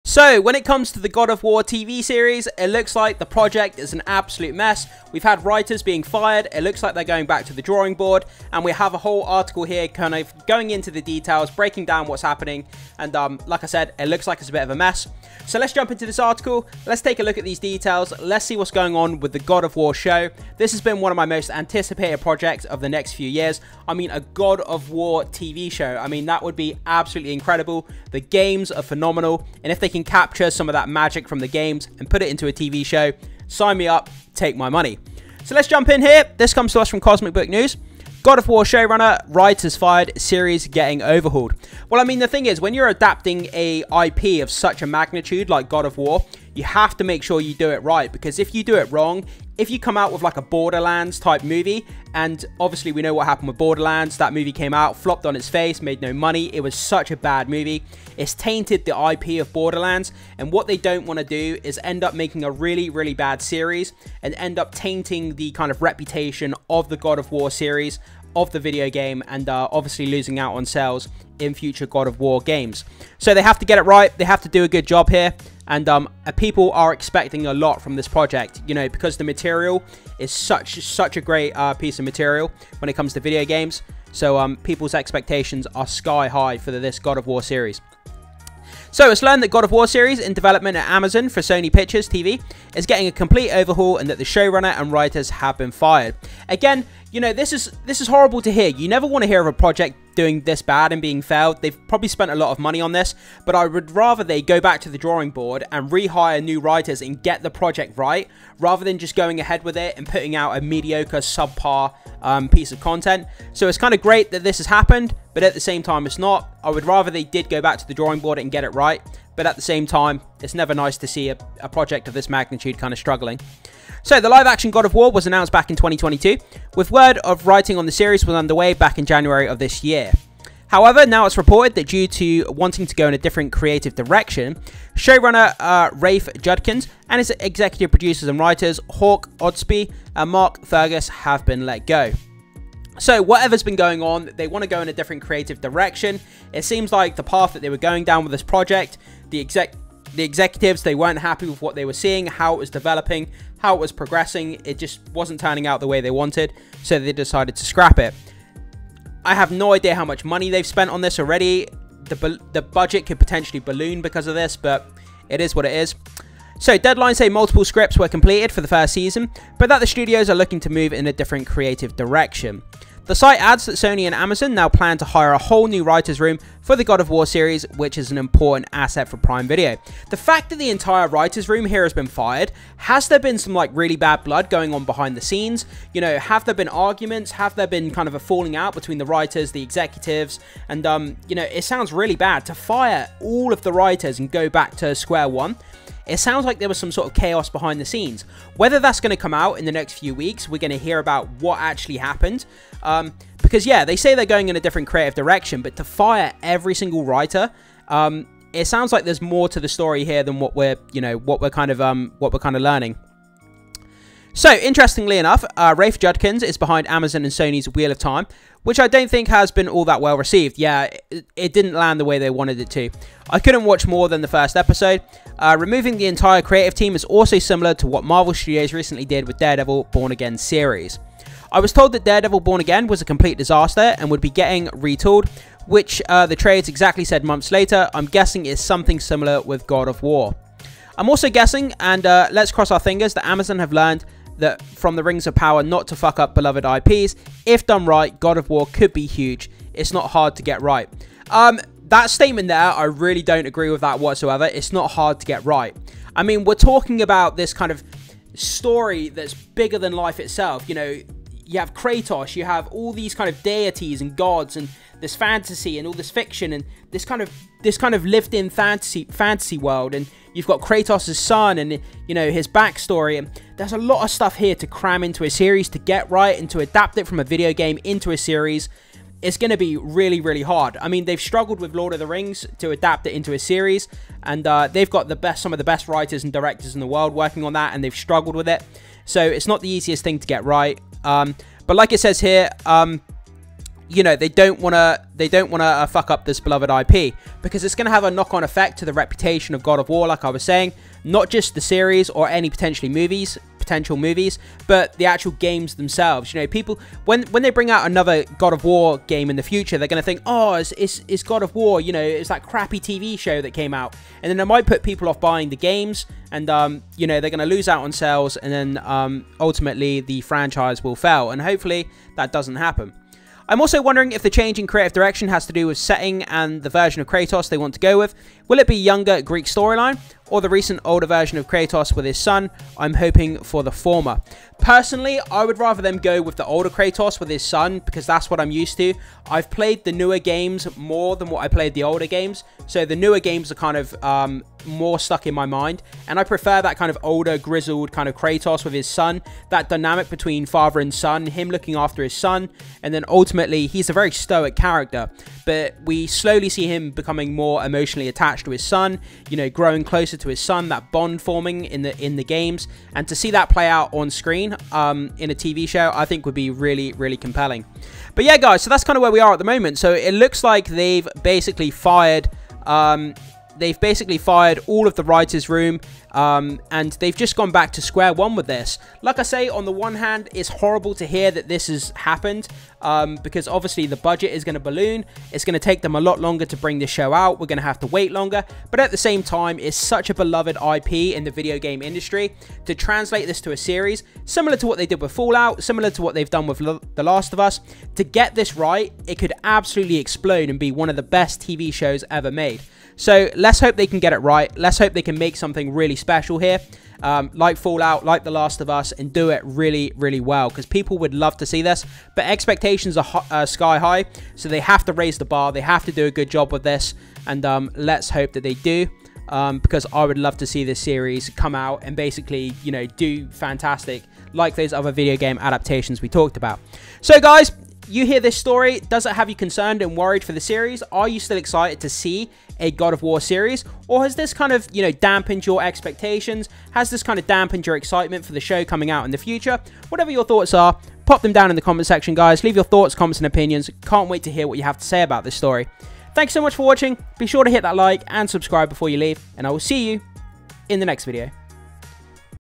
God of War Series in Chaos? Showrunner & Writers Fired as Major Overhaul Begins!
The God of War TV series is facing major shakeups! In shocking news, the showrunner and writers have been fired, and the entire series is reportedly being overhauled. What does this mean for the highly anticipated adaptation of the beloved video game? Could the changes improve the series, or is this a sign of trouble ahead? Join us as we dive into the latest updates, rumors, and what this means for the future of God of War on the small screen!

Timestamps
00:00 - 01:28 - Introduction
01:28 - 03:33 - Overhaul
03:33 - 05:23 - My Thoughts
05:23 - 06:07 - Creative Direction
06:07 - 07:06 - High Expectations
07:06 - 07:31 - Your Thoughts!?